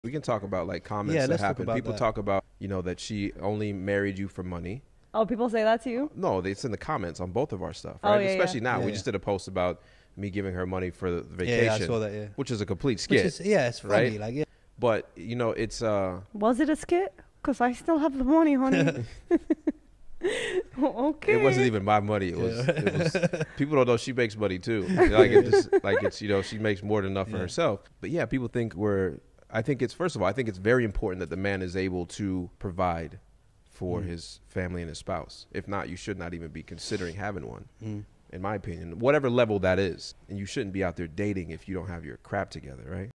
Max on people saying Maisvault married him for his money
AJ is joined in the studio by

 Powered by VUUZLE